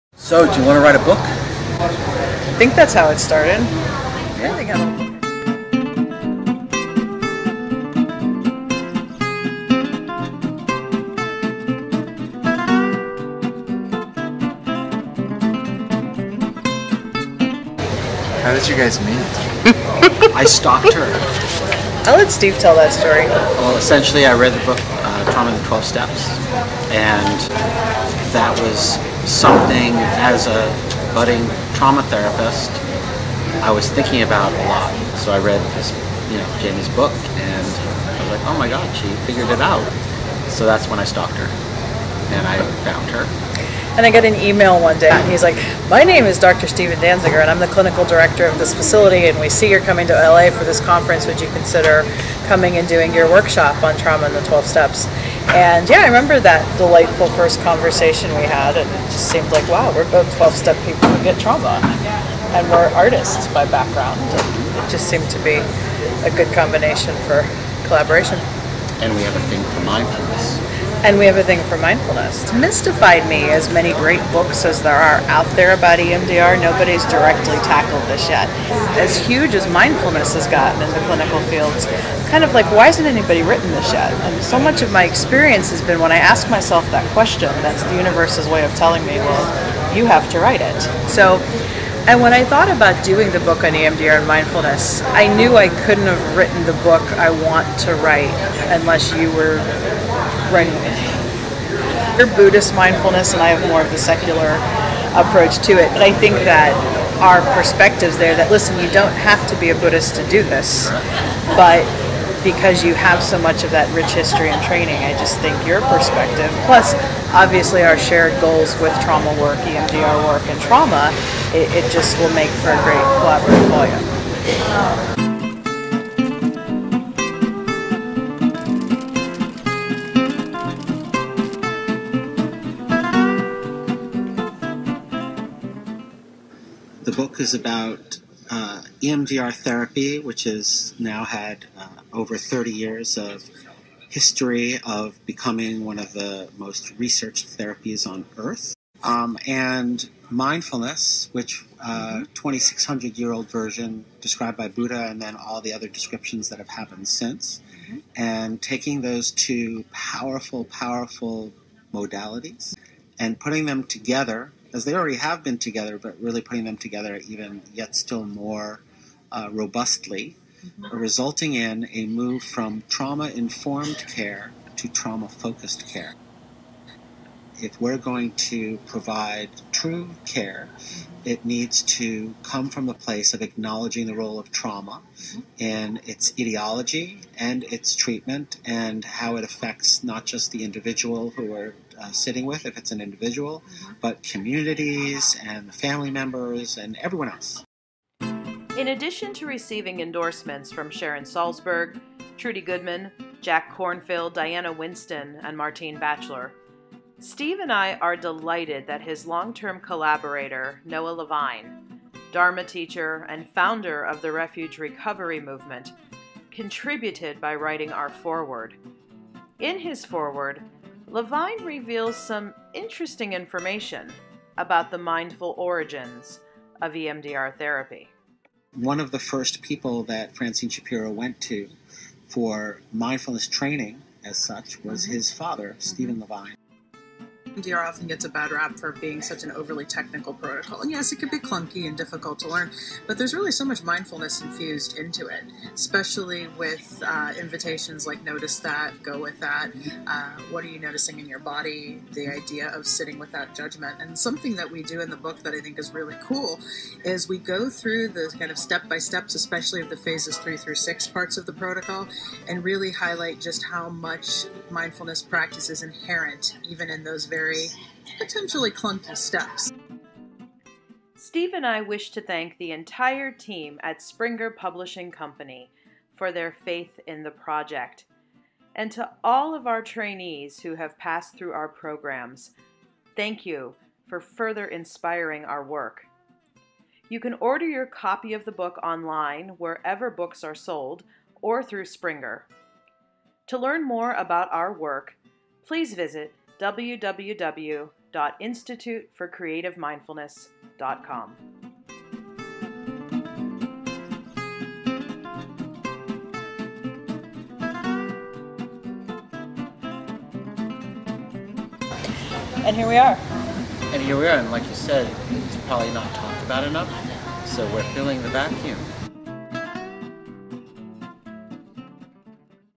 EMDR Therapy & Mindfulness for Trauma-Focused Care: SHORT FILM
Discover the origin story of "EMDR Therapy & Mindfulness for Trauma-Focused Care," a new book by Dr. Jamie Marich and Dr. Stephen Dansiger, coming November 17, 2017 with Springer Publishing Company.

To learn more about the team behind the work, go